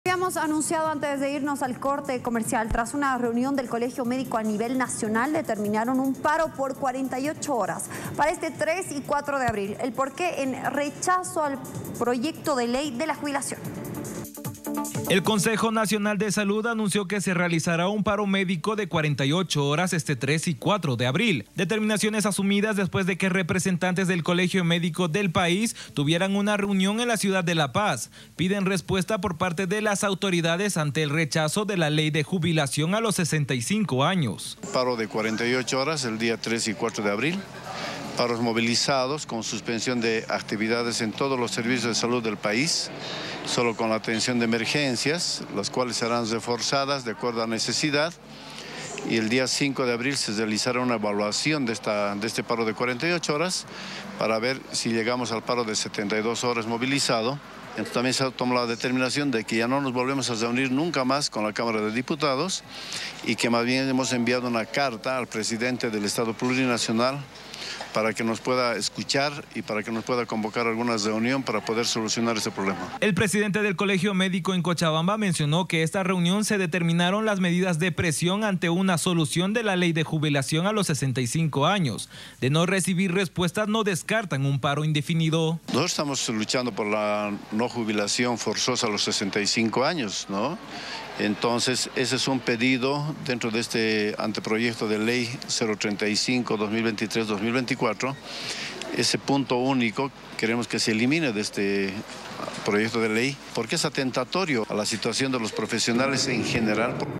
Tras reunión médico determinan paro de 48 horas a nivel nacional
Después de una reunión del Colegio Médico a nivel nacional, se ha decidido llevar a cabo un paro de 48 horas para el 3 y 4 de abril, en protesta contra el Proyecto de Ley de Jubilación.